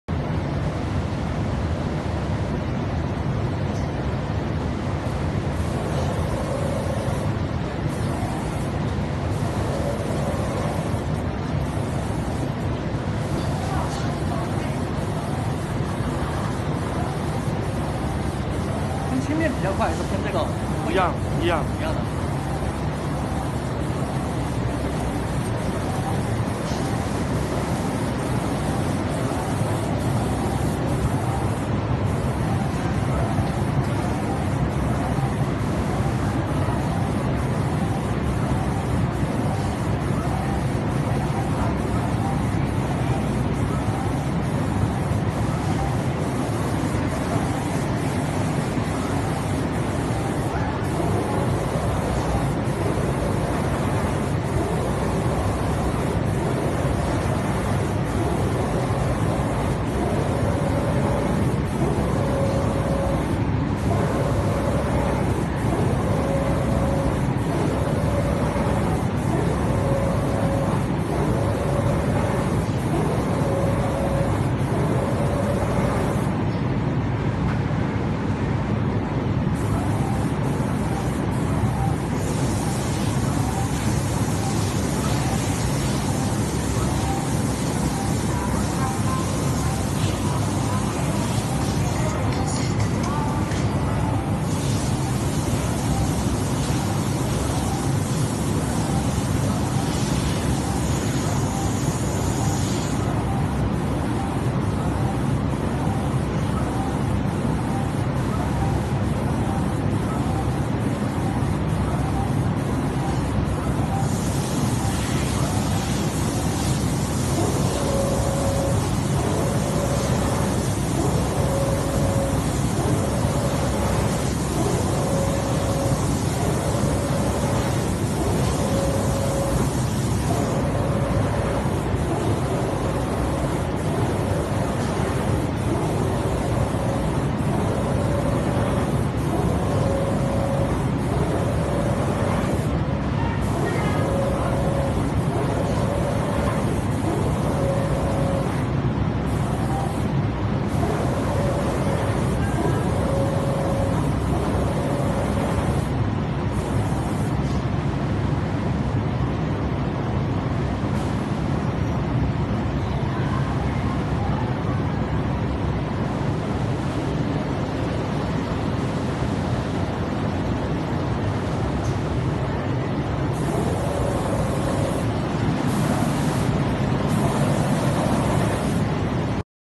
5 AXIS CNC DOOR SPRAY PAINTING MACHINE
Automatic automatic spraying equipment scheme and technical description for automatic wooden doors.